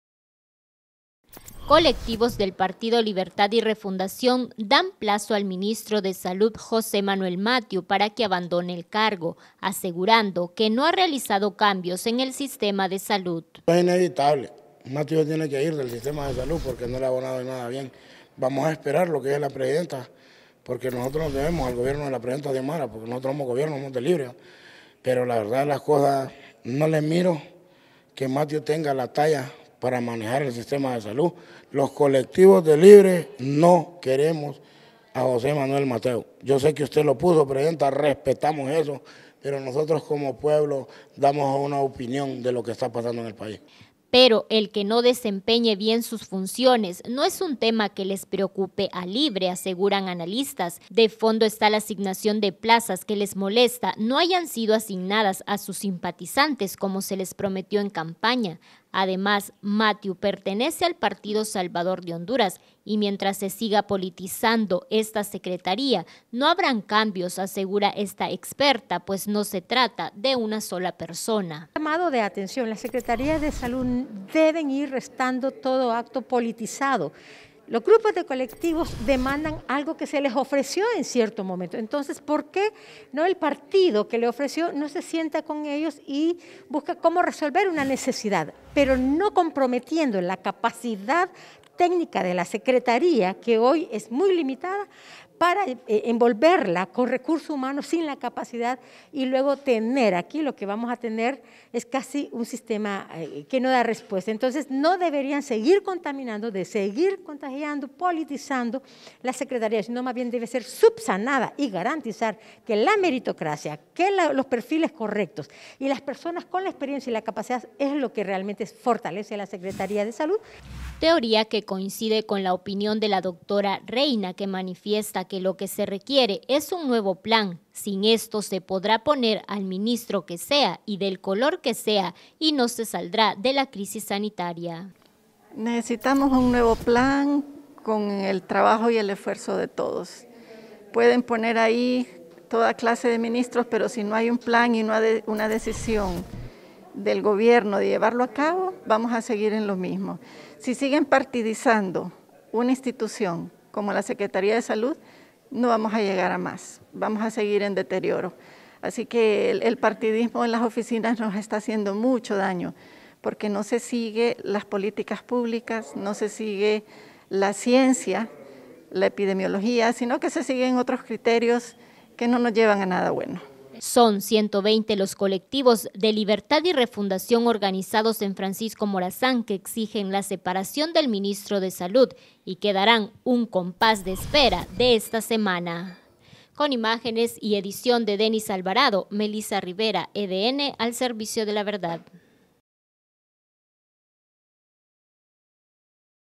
Colectivos de Libre dan plazo para destitución de ministro de Salud | EDNTV
Los colectivos de libre han vuelto a la carga y piden la destitución del ministro de Salud, José Manuel Matheu dando esta semana como plazo.